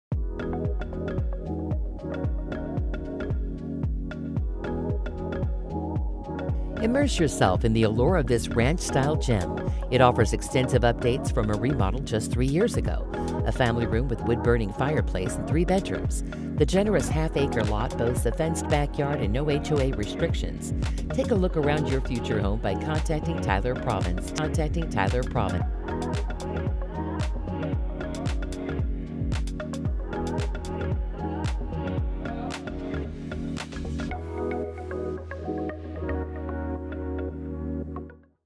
12440 E 256th Street Cicero, IN | MLS# 21942482
12440 E 256th Street, Cicero, IN

Immerse yourself in the allure of this ranch-style gem, located in the desirable Hamilton Heights School district. Uniquely, there are no HOA restrictions, and the property was extensively remodeled just 3 years ago. The backyard is fenced, with an RV plug on the garages exterior and ample RV parking space next to the house. The garage boasts 220V outlets, a handy addition for any DIY enthusiast. Inside, youll find 3 bedrooms and 2 full baths, a living room, and a family room with a wood-burning fireplace. All of this sits on a generous .5-acre lot, making it the perfect place to call home.